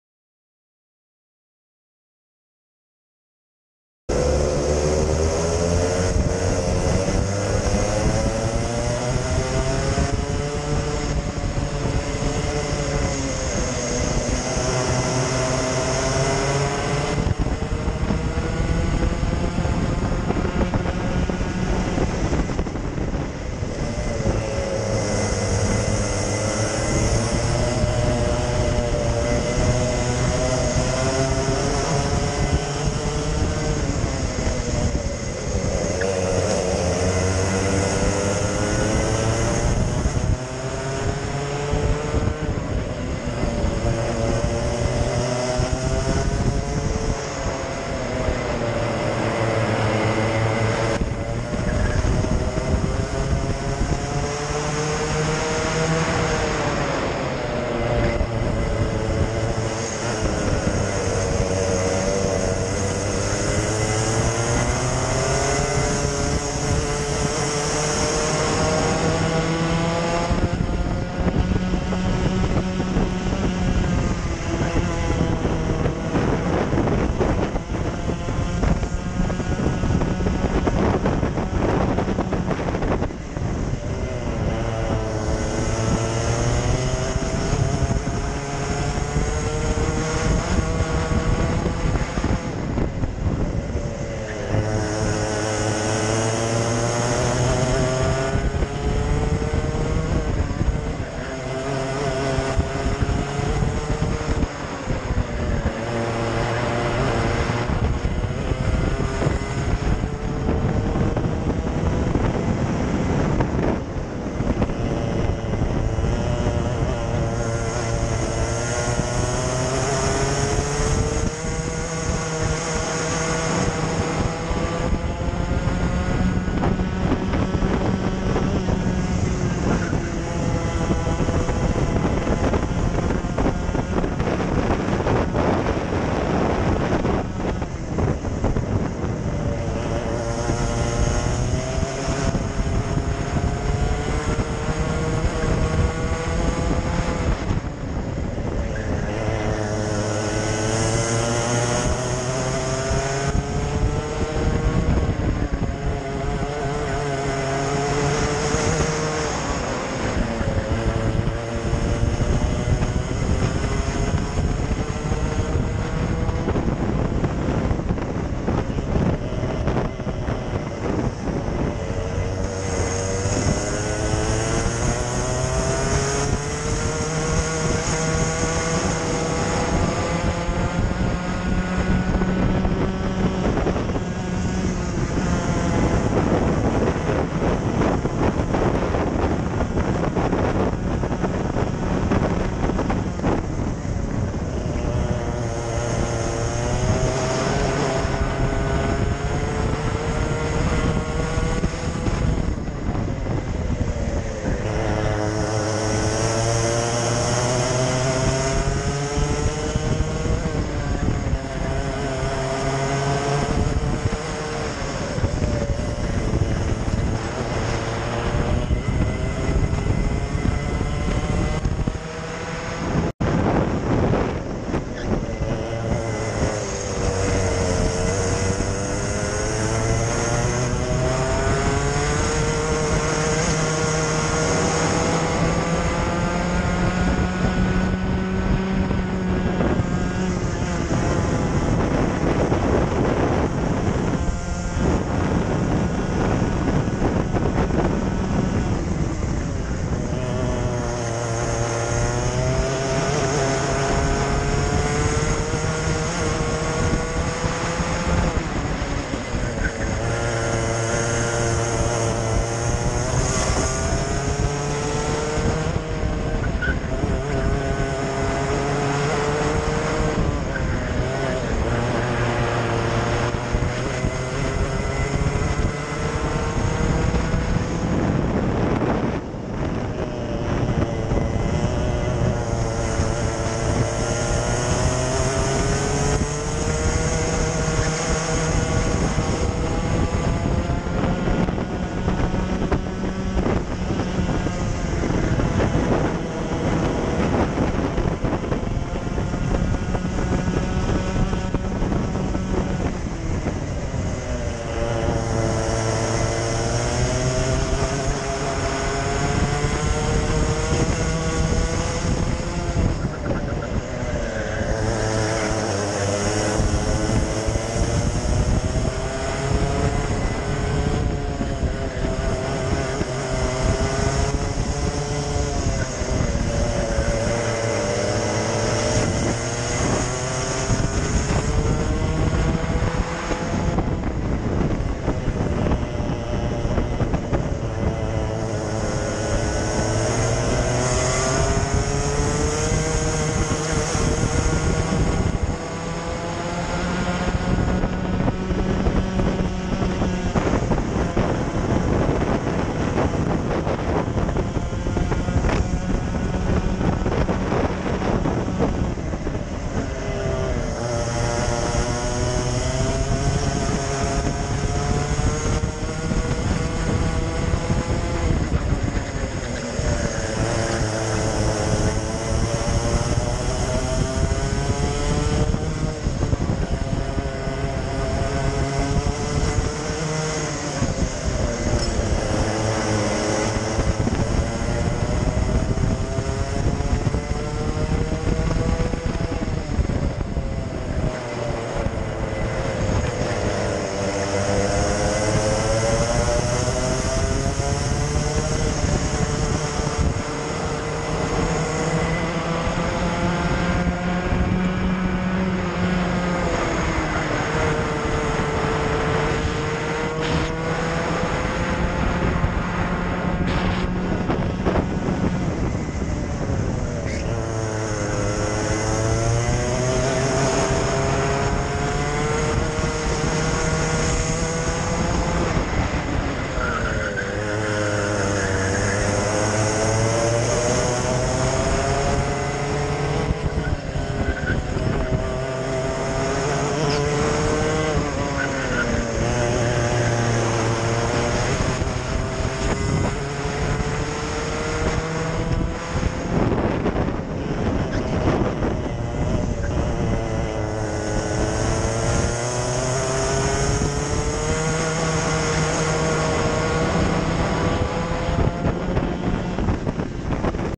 Club 100 2019 Heavyweight Sprints round 3 - heat 1: Whilton Mill
The opening heat race from round 3 of the Club 100 Heavyweight Sprints Championship from Whilton Mill.

Sub-class: Class 1
Kart number: 12
Start: 10th
Result: 8th